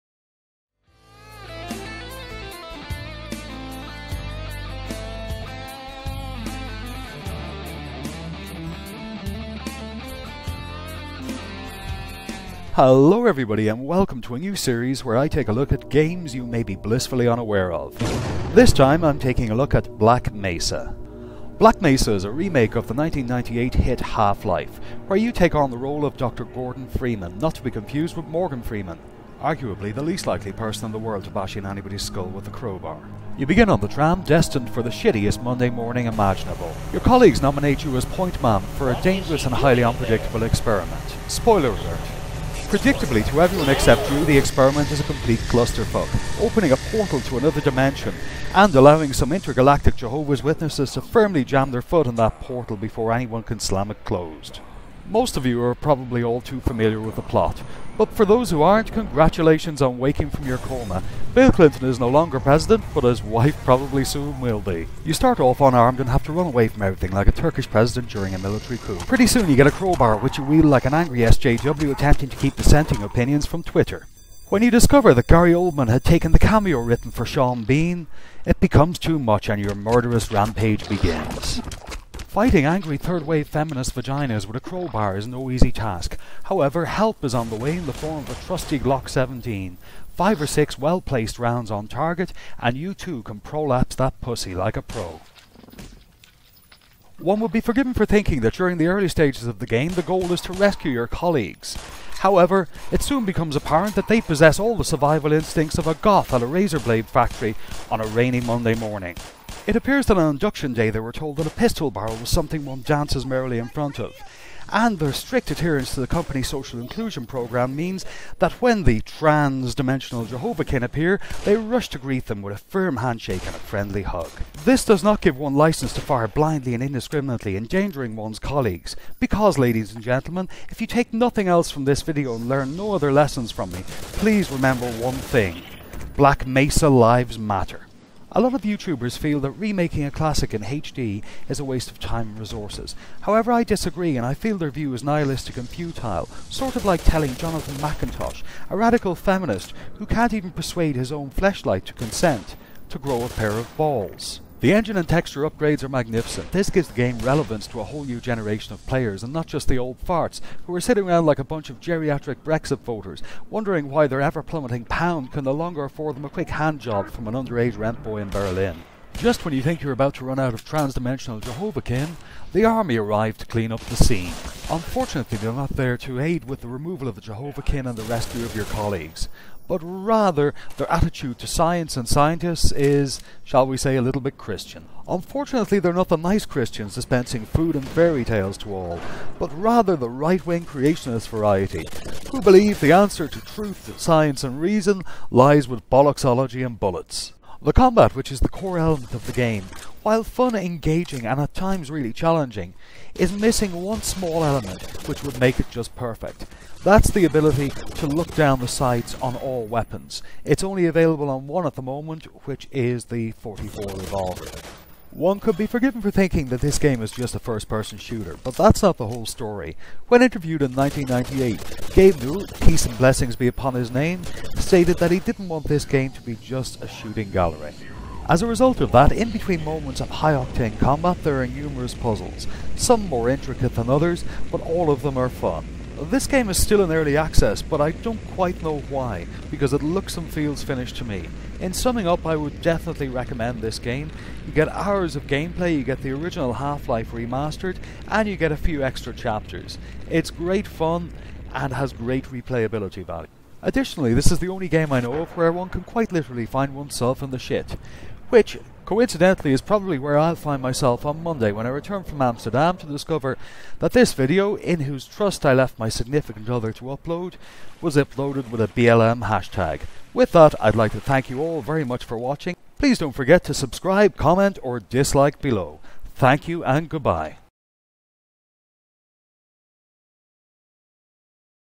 Here's One You Probably Missed - Black Mesa Review!
New Series! - Taking a look at games which have been around for a little while, but have to a greater or lesser extent have fallen under the radar.